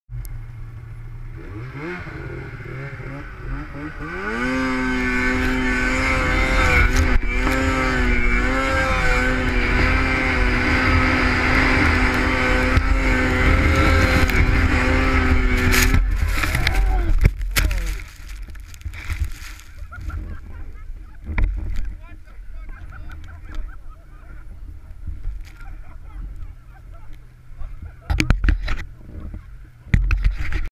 Mustache Mayhem
The man the myth the legend was spotted at Crooked Creek Guest Ranch a few days ago. We hog tied him and stole this GoPro footage. This is only a sneak peak into his insane riding skills.